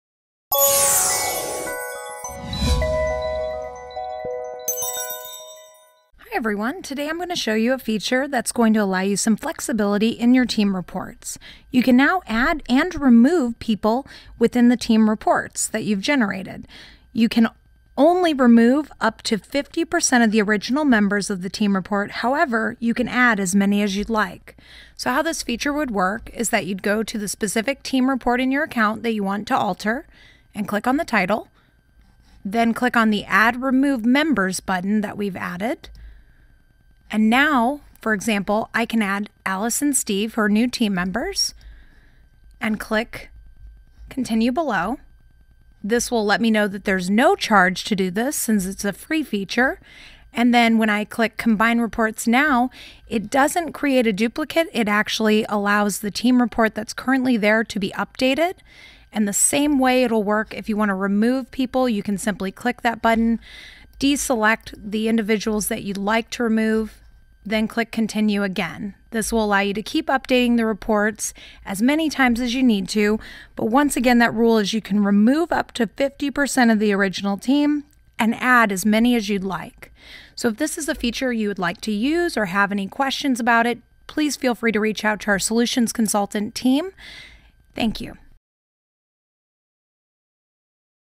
Learn how to use add/remove team member feature on IDS
Hi everyone today I'm going to show you a feature that's going to allow you some flexibility in your team reports. You can now add and remove people within the team reports that you've generated. You can only remove up to 50% of the original members of the team report. However, you can add as many as you'd like so how this feature would work is that you'd go to the specific team report in your account that you want to alter and click on the title then click on the add remove members button that we've added and now, for example, I can add Alice and Steve her new team members and click continue below this will let me know that there's no charge to do this, Since it's a free feature and then when I click combined reports now it doesn't create a duplicate it actually allows the team report that's currently there to be updated and the same way it'll work. If you want to remove people you can simply click that button deselect the individuals that you'd like to remove then click continue again this will allow you to keep updating the reports as many times as you need to but once again that rule is you can remove up to 50% of the original team and add as many as you'd like so.